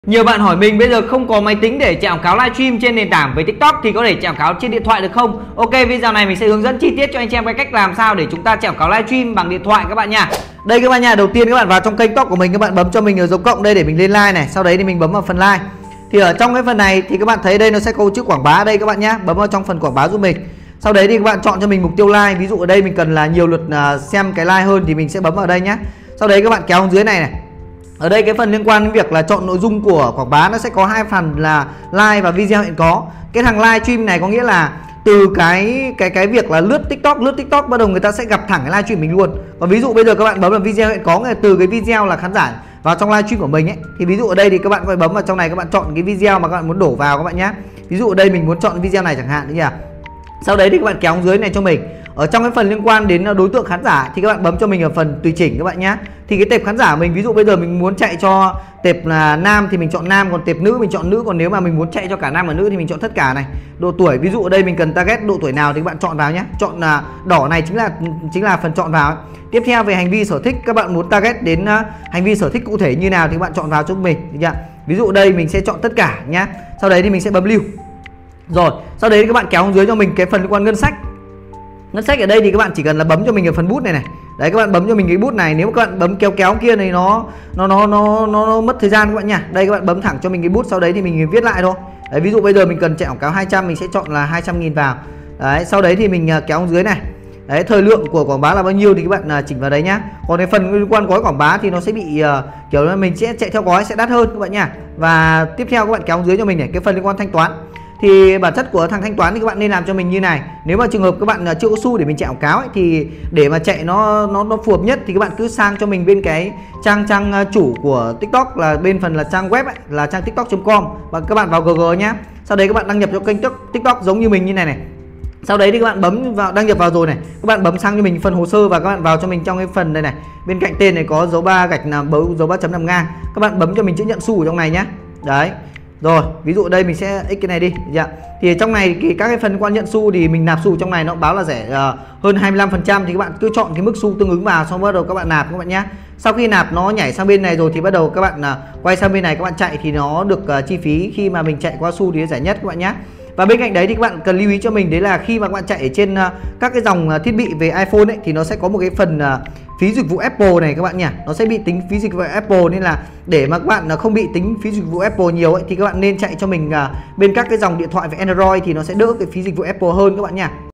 Cách chạy quảng cáo tăng mắt xem livestream tiktok bằng điện thoại | Đàm Văn Tuấn
Video tiếp theo Tuấn sẽ chia sẻ về cách chạy quảng cáo livestream tiktok shop.

★ Phone/Zalo : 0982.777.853    (Zalo 2: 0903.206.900)